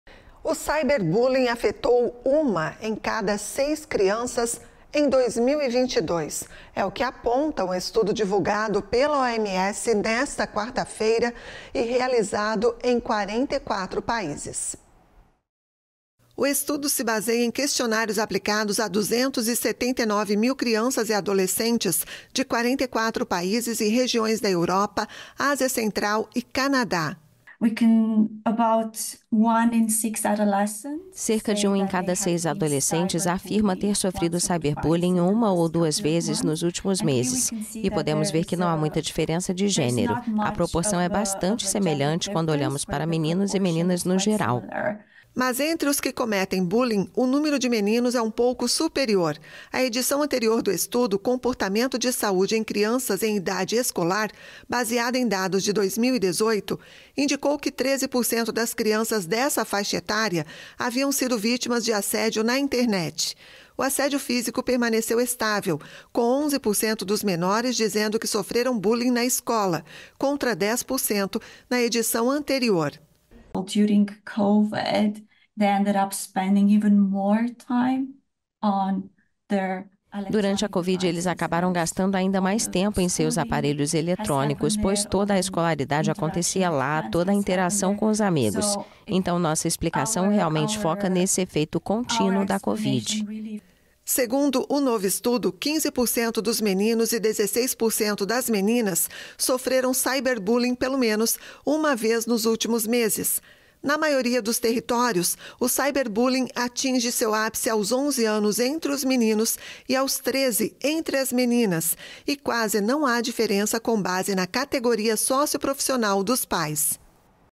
Cyberbullying afetou 1 em cada 6 crianças em 2022, diz OMS
O cyberbullying afetou uma em cada seis crianças em 2022. É o que aponta um estudo divulgado pela Organização Mundial da Saúde (OMS). Cerca de 279 mil crianças e adolescentes de 44 países participaram da pesquisa.

  / pixtvhd
  / pixtvhd
  / pixtvhd

🅿️ℹ️❎
PIX NEWS